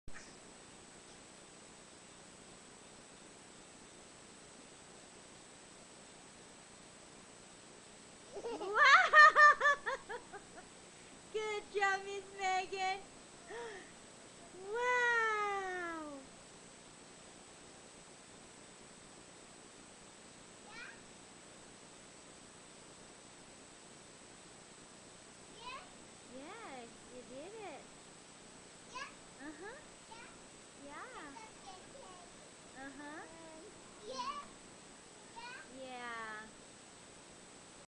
Monkey Bars Megan @ 21 months
Megan first burst on the scene with her incredible grip - she now is displaying her new move in her gymnastic bag of tricks!